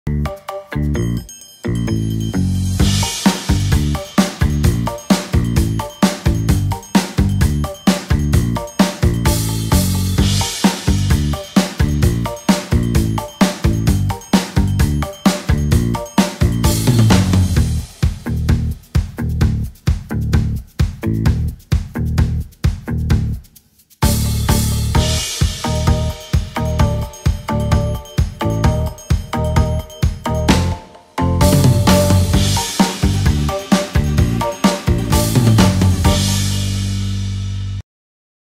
Super heated water can explode outside of microwave
When heating in the microwave, there's a chance it can become over heated. This is dangerous, as the water has been warmed past its boiling point and even a slight jostle can release the gases and cause it to explode.